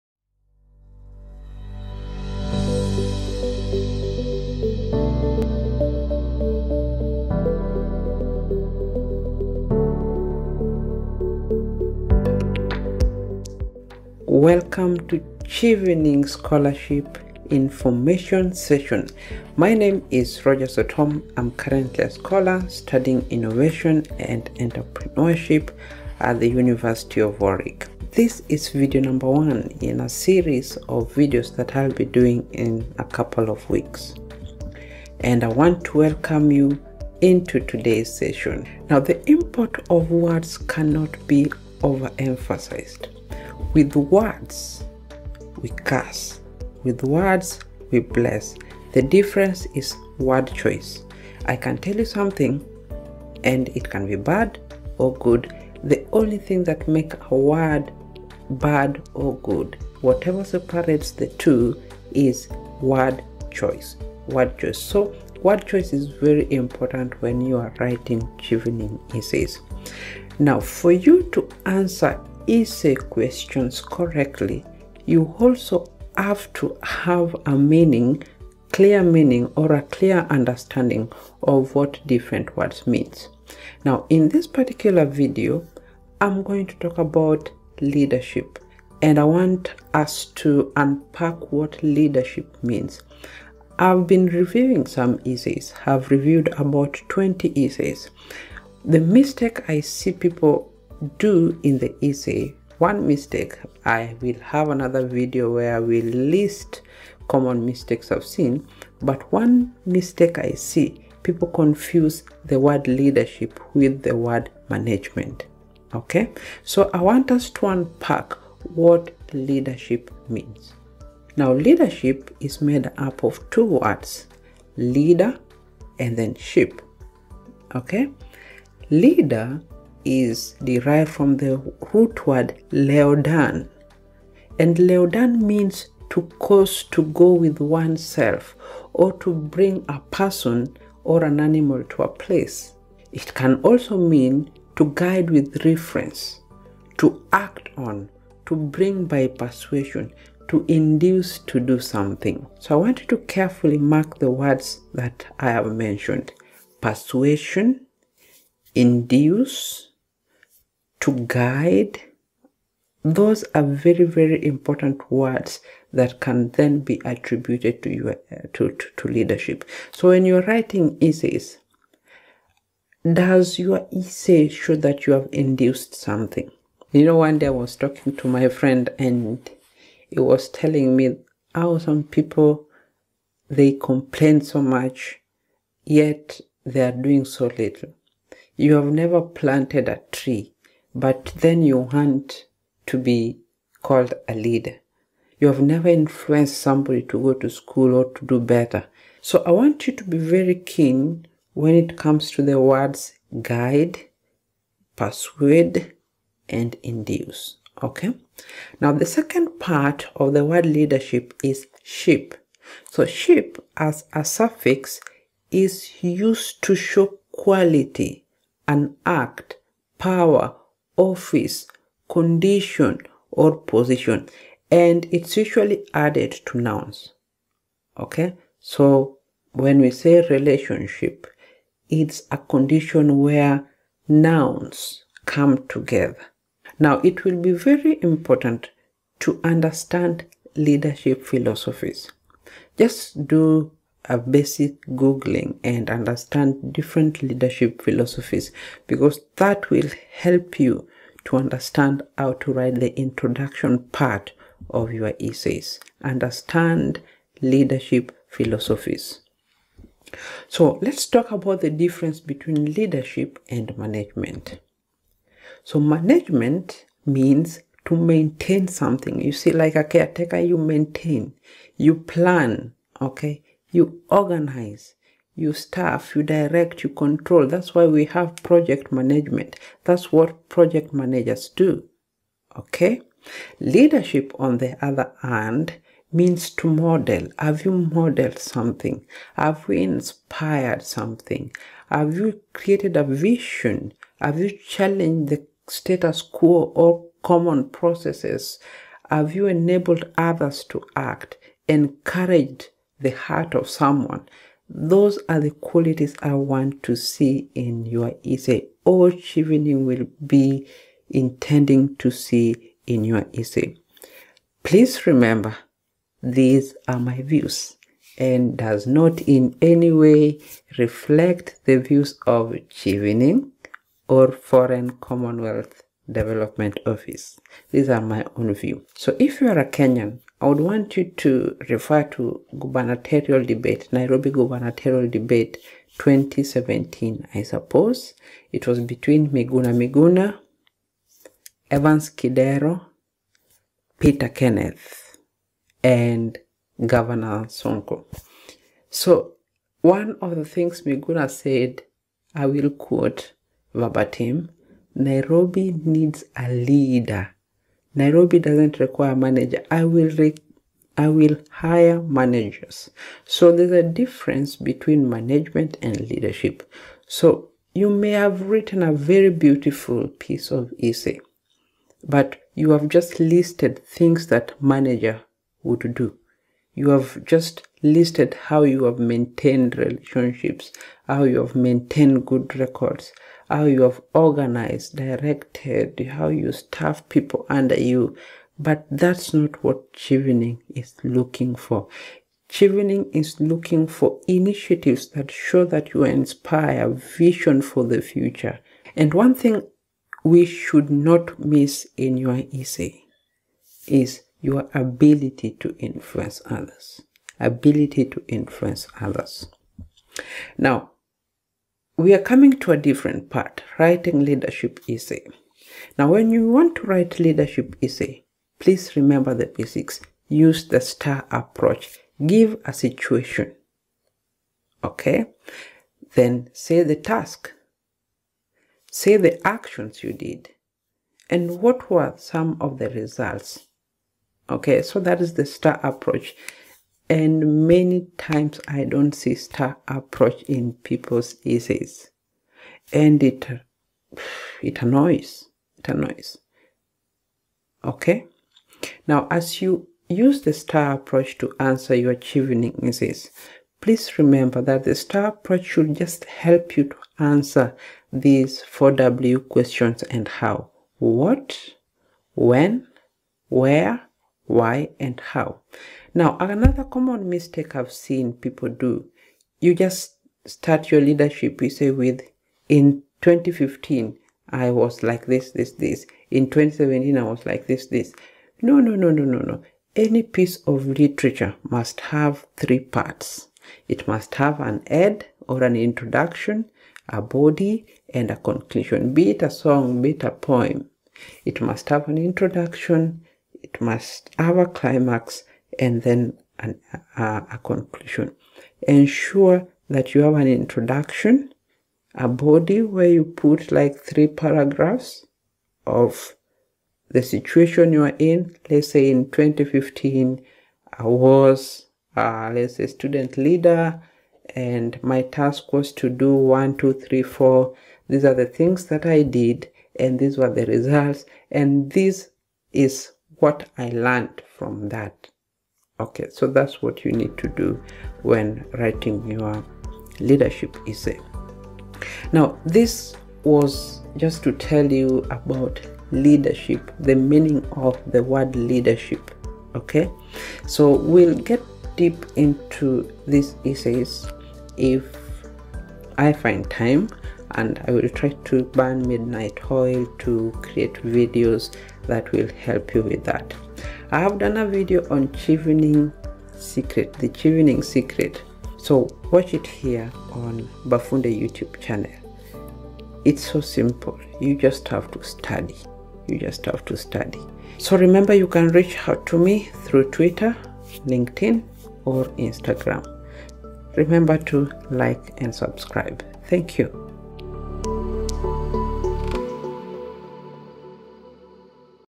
Understanding chevening Leadership essay
Many people confuse leadership with management. In this video, I explain what leadership entails and what is expected from you when you write chevening scholarship2022 leadership essay.